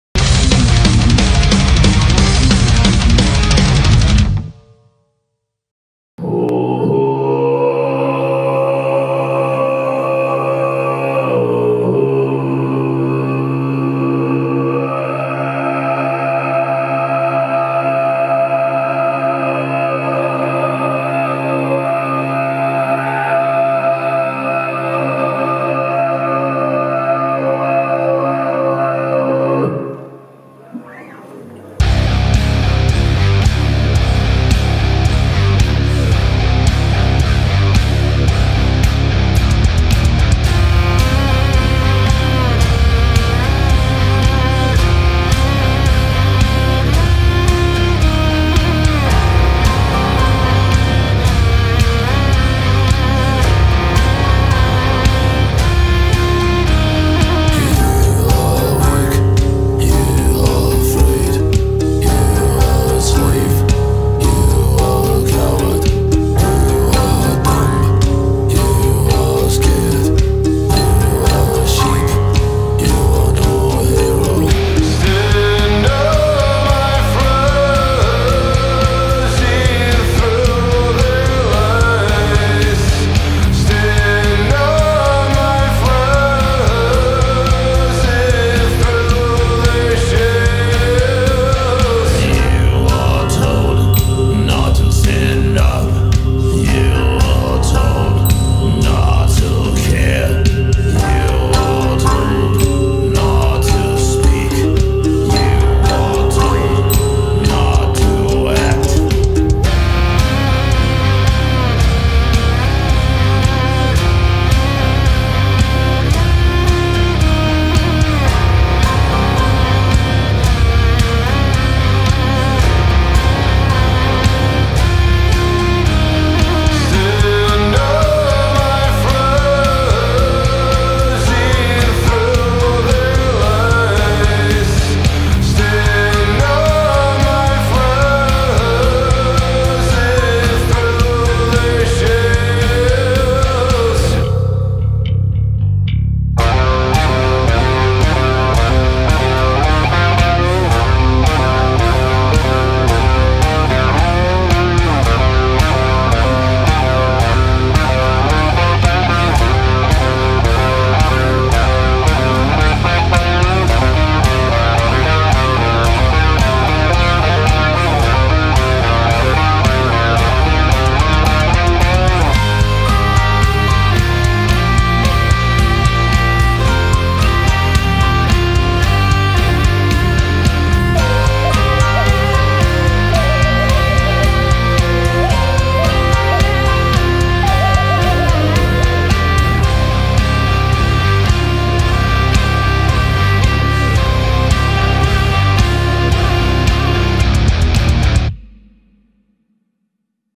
Independence Day @ Brest,​ with guest Möhrkvlth​
Tengger Cavalry live in Brest, Rock Cirkus Bar​ with guest band Möhrkvlth​

Video by Metal Breton​